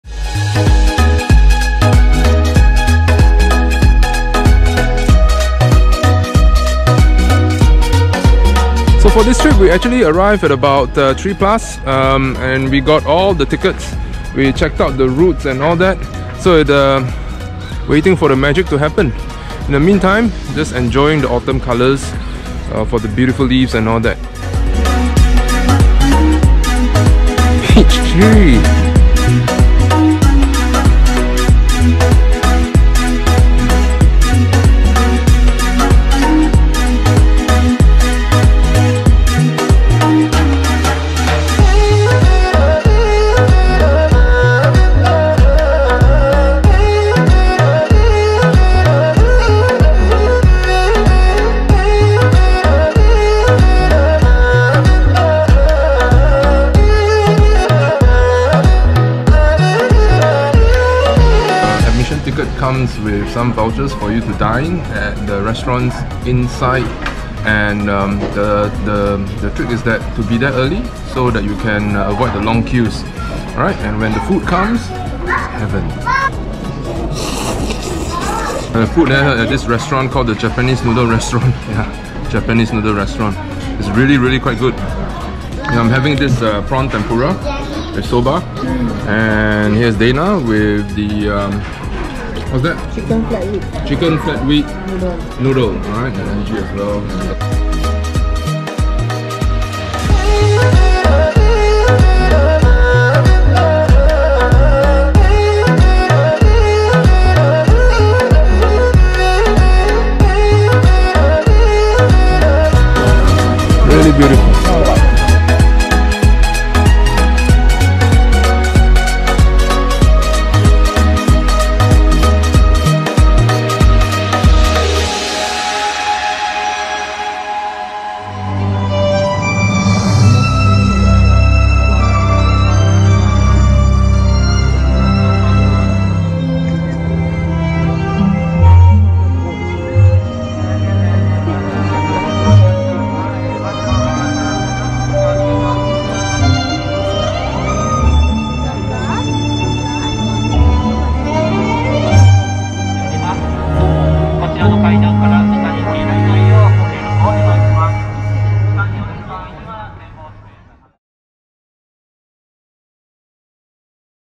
Stunning Nabana No Sato - Nagoya Japan
Easily the most stunning Winter Illumination we have ever seen!  This flower park is huge and takes on a different 'life' when night falls during winter.  We were lucky that when we visited in early December, the autumn leaves were still in 'full bloom' giving us the rare opportunity to take countless family photos with spectacular fall colours!
When night fell, the lights took over and the place took a life of its own.  There are so many experiences not to be missed within the flower park some of which I shared in the video.